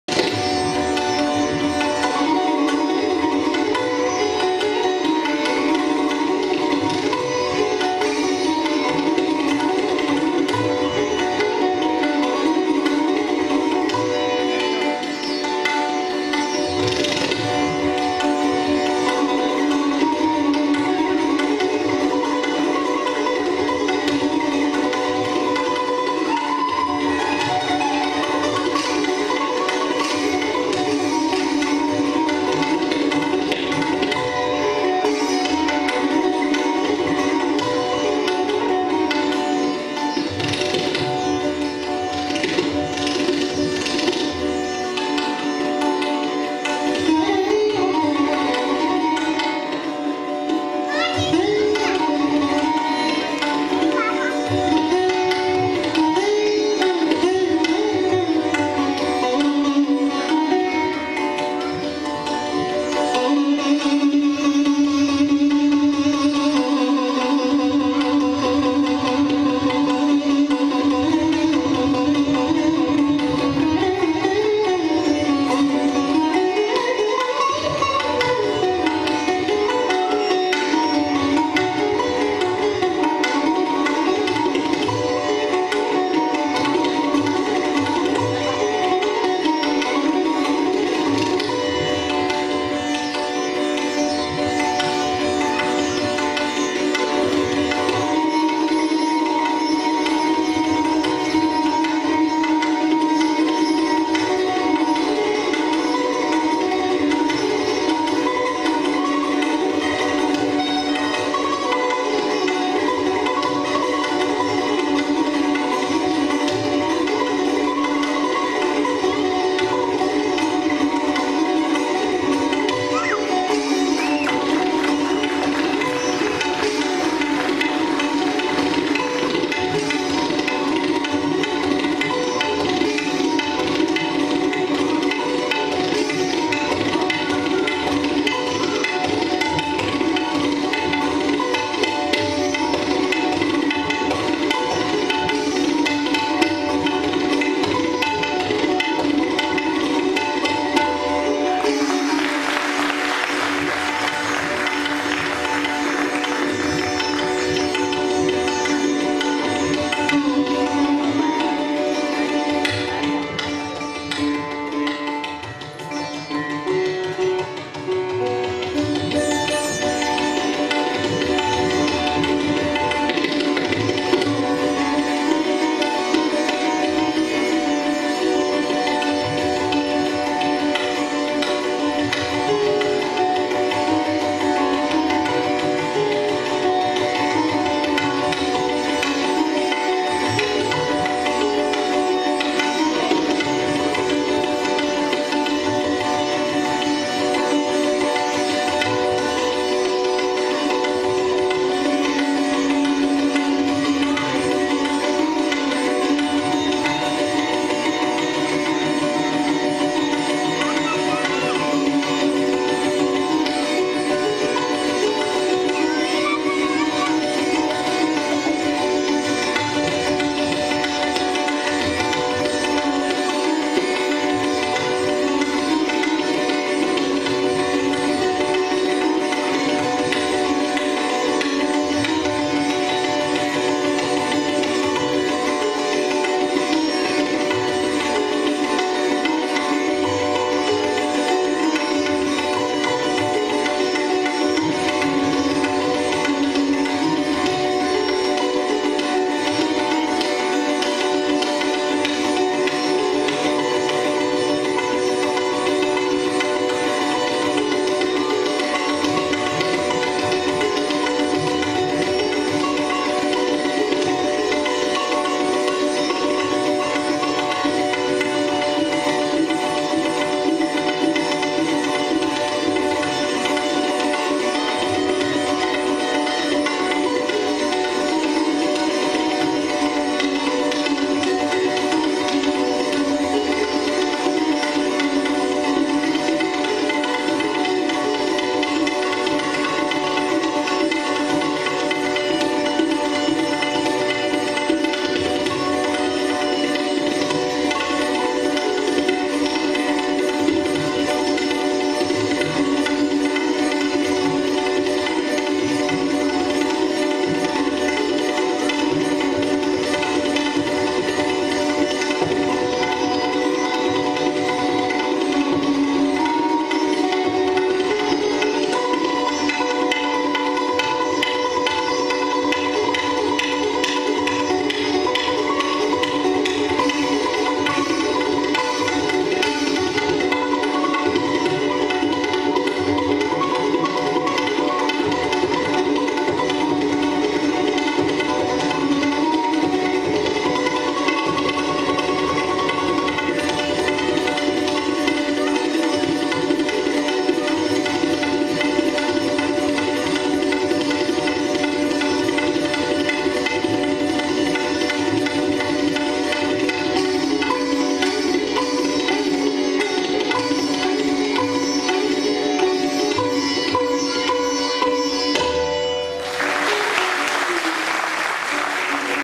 Shafique Khan and Rajendra Nakod, Raag Marwa 5, Drut Teen-Taal
PROGRAMME HELD ON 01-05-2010 AT JSS AUDITORIUM BANGALORE BY SWARAMADHURI ACADEMY